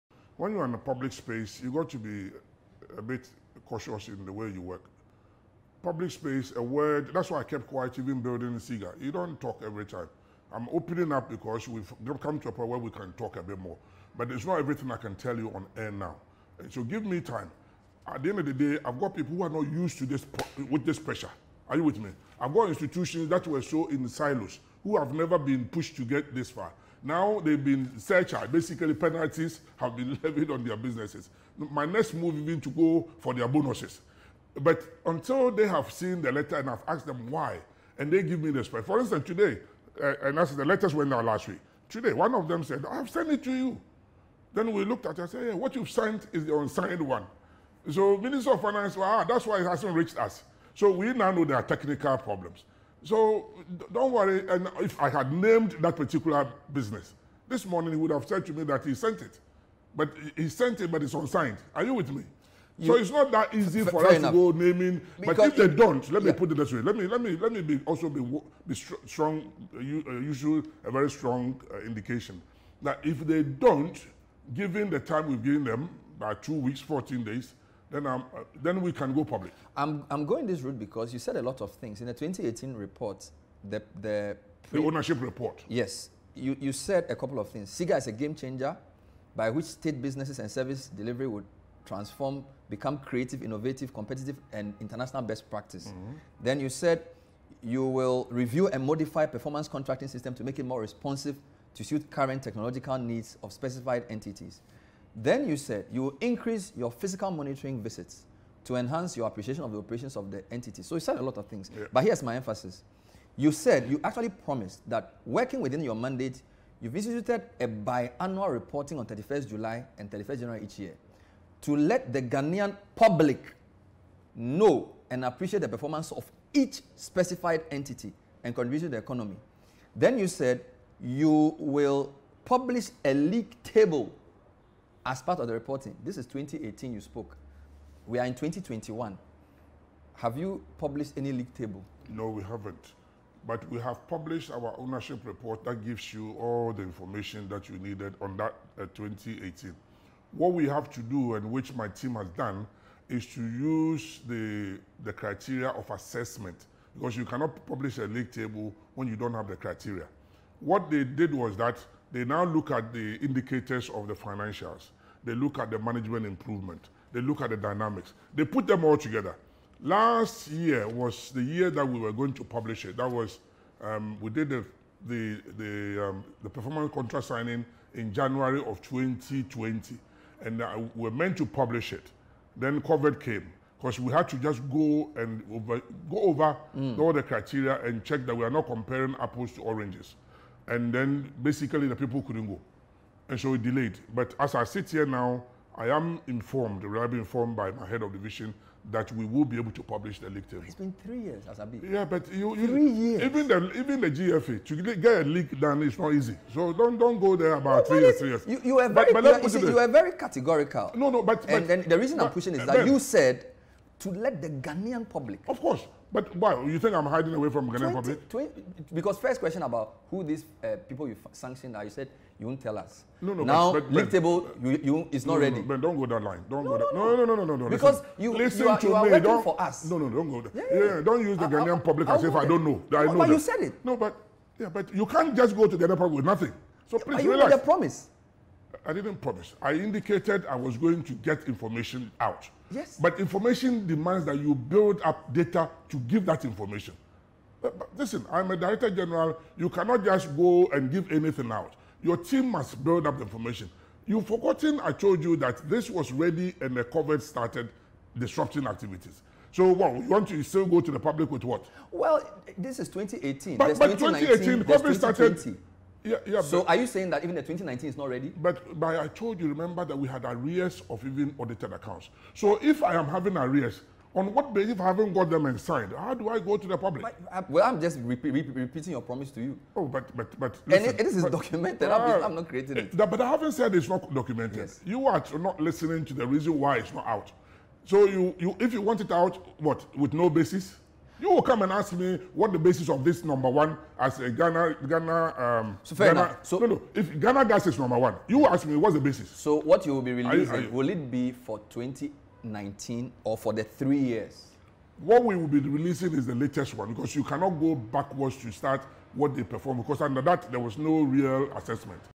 SIGA boss explains delay in releasing league table to assess State-Owned Enterprises
The Director-General of the State Interests and Governance Authority (SIGA), Stephen Asamoah-Boateng, explains why the proposed 'league table' to assess State-Owned Enterprises has delayed.